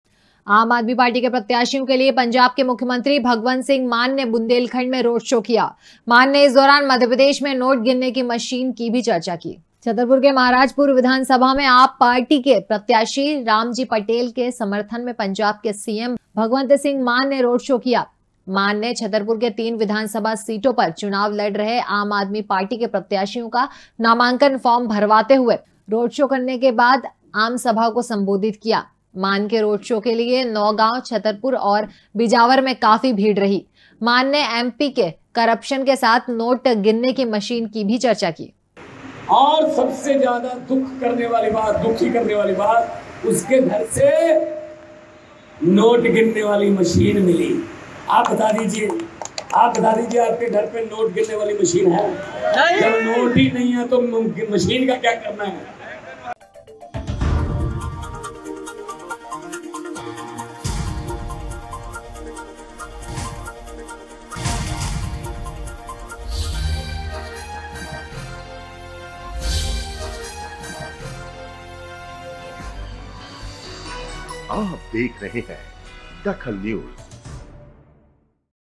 DAKHAL NEWS बुंदेलखंड में भगवंत मान का रोड शो
बुंदेलखंड में भगवंत मान का रोड शो
मान ने की नोट गिनने की मशीन की चर्चा